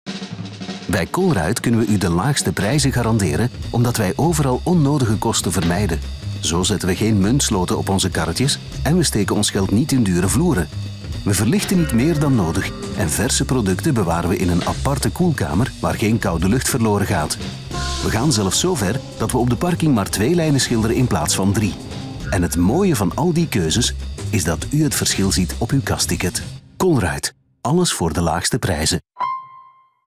Gegarandeerd de laagste prijzen: deel 1
Bij Colruyt kunnen we de laagste prijzen garanderen door onnodige kosten te vermijden. Enkele voorbeelden ...